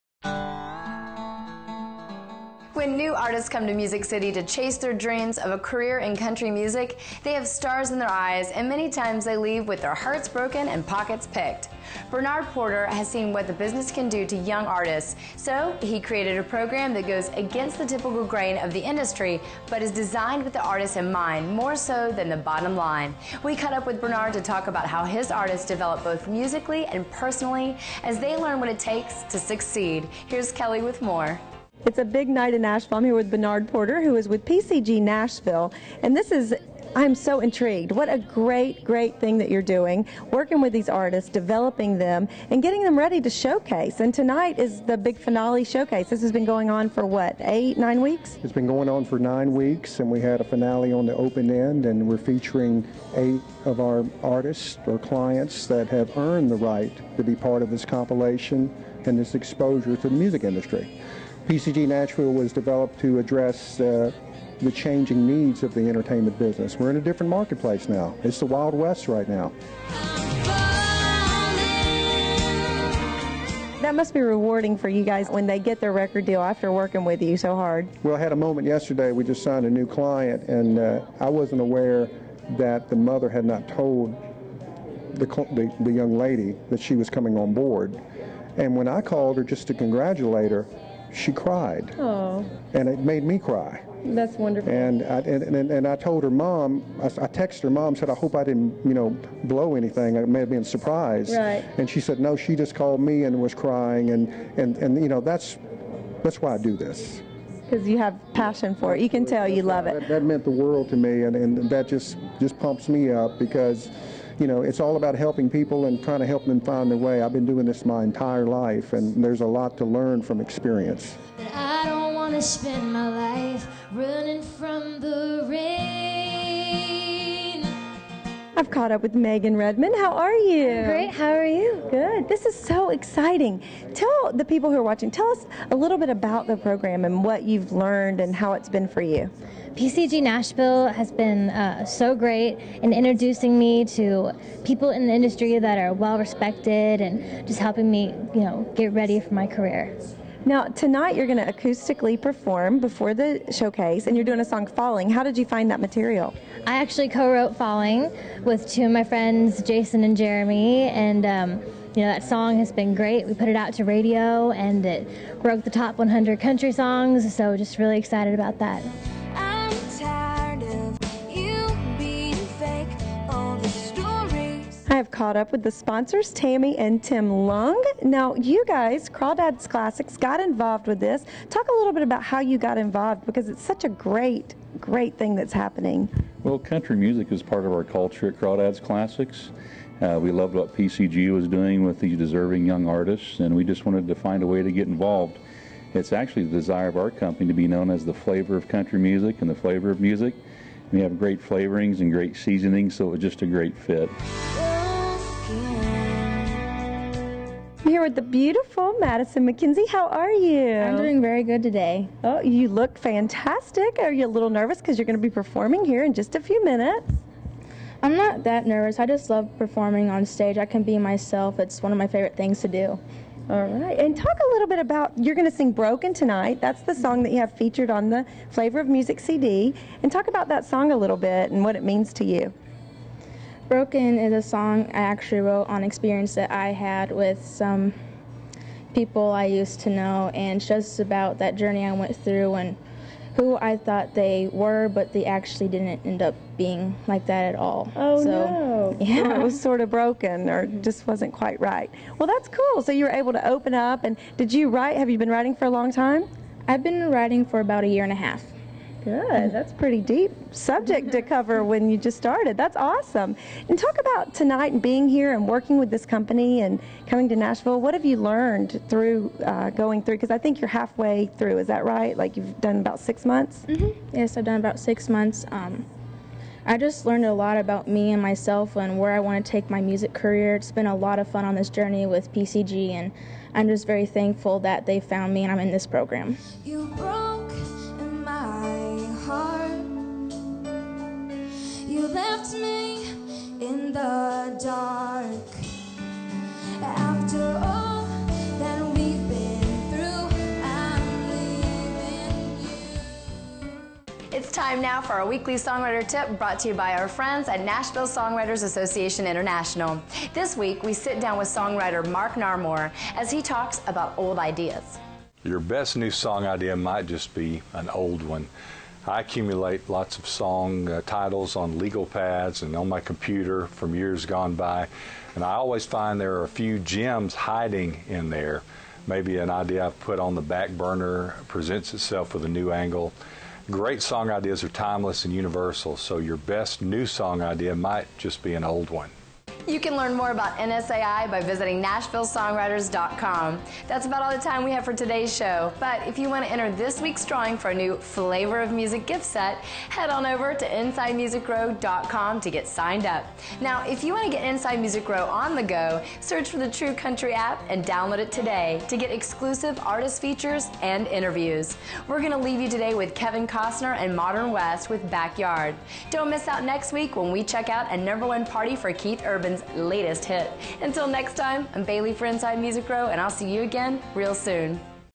Flavor of Music - Inside Music Row 1254
When new artists come to Music City to chase their dreams of a career in country music, they have stars in their eyes and many times they leave with their hearts broken and pockets picked.  Bernard Porter has seen what the business can do to young artists, so he's created a program that goes against the typical grain of the industry, but is designed with the artist in mind more so than the bottom line.  We caught up with Bernard to talk about how his artists develop both musically and personally as they learn what it takes to succeed.  Here's Kelly with more.